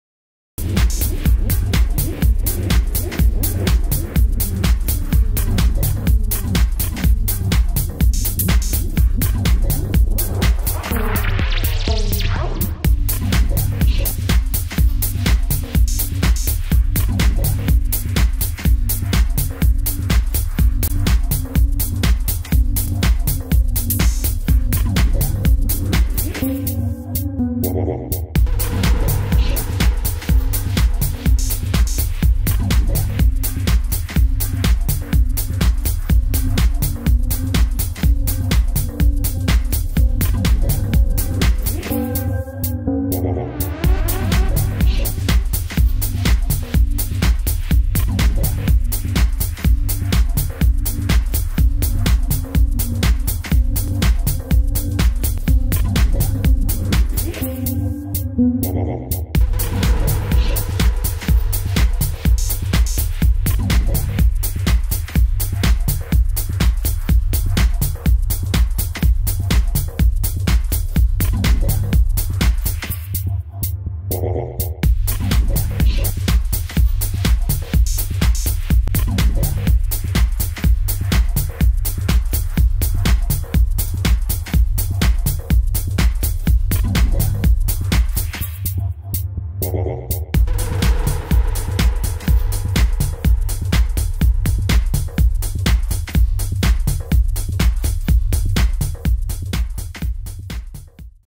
Pasarela Jorge Duque Caliexposhow 2016 - Adelanto
Con su nueva colección, el diseñador Jorge Duque, hace un homenaje al arte decorativo y popular nacido a principios del siglo XX en Buenos Aires. Más detalles de este gran desfile y PASARELA COMPLETA muy pronto por

Bogotá - Colombia